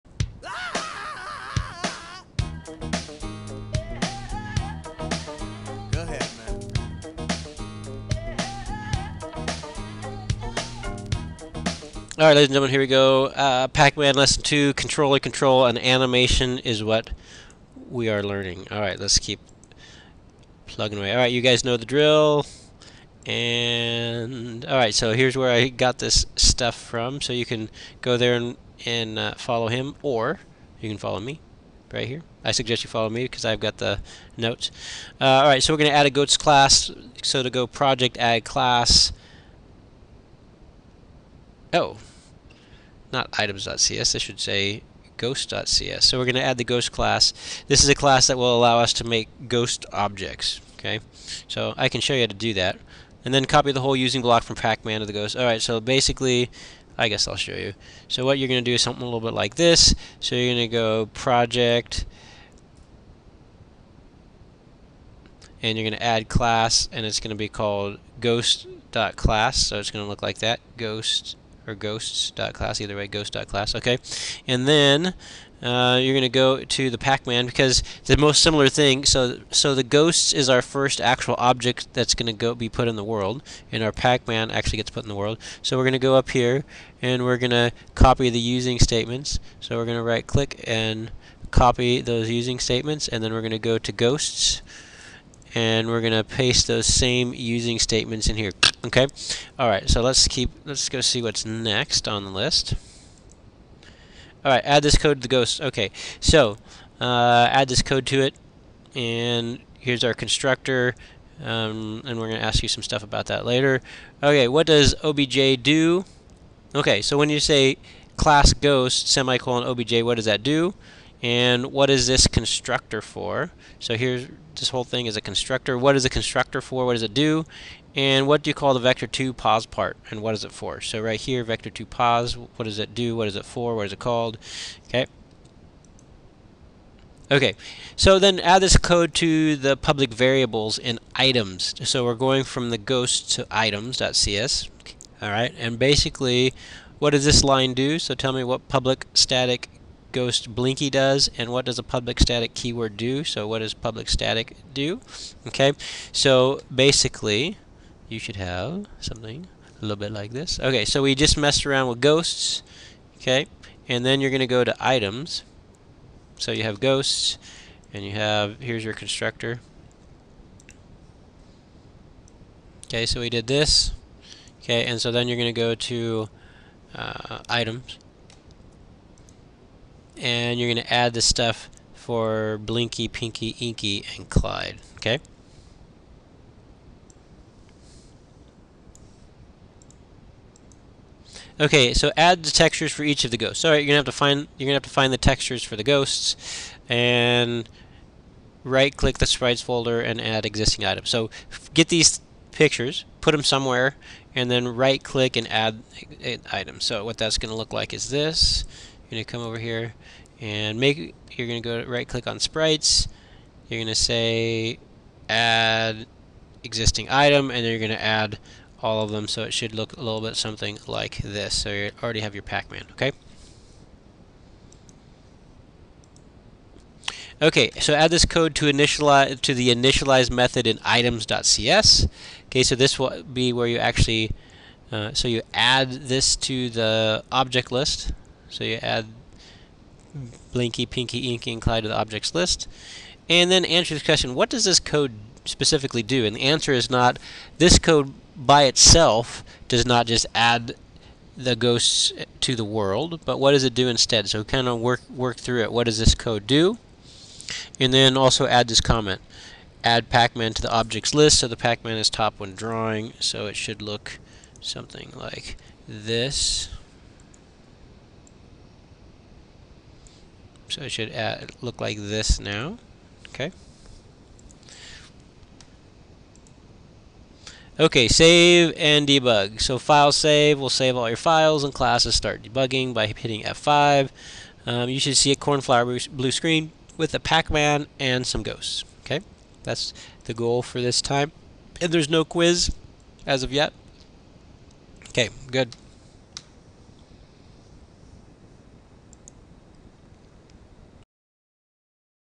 SpielPart1Lesson2Pacman
High school lesson plan on using c# and XNA to create games.